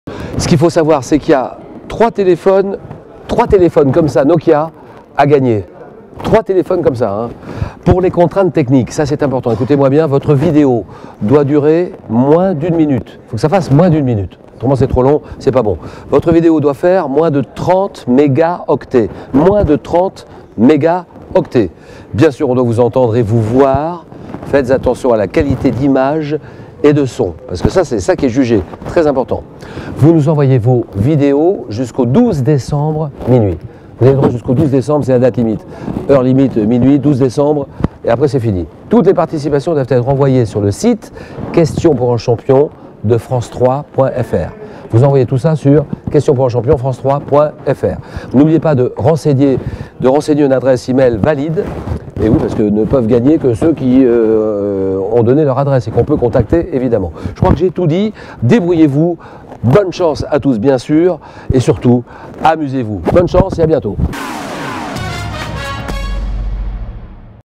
CartonneAvecMaQuestion - Julien Lepers vous explique les règles du concours !
Pour fêter le million de fans sur la page Facebook de Questions pour un Champion, Julien Lepers a décidé de lancer le concours CartonneAvecMaQuestion !

Dans cette vidéo, Julien Lepers vous explique quelles conditions il faut absolument remplir pour pouvoir participer à ce concours !

Vous souhaitez participer au concours ?
Merci à tous pour votre fidélité et amusez-vous !